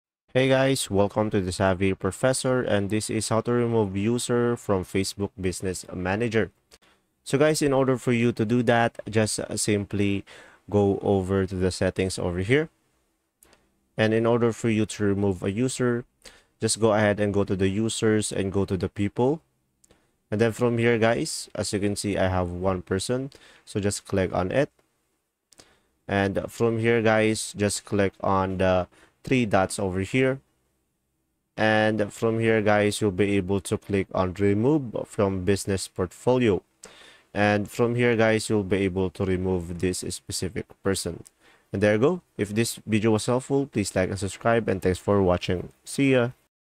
How To Remove User From Facebook Business Manager(How Do I Remove Someone From FB Business Manager?)
How To Remove User From Facebook Business Manager(How Do I Remove Someone From FB Business Manager?). In this video tutorial I will show you how to remove user from Facebook Business Manager.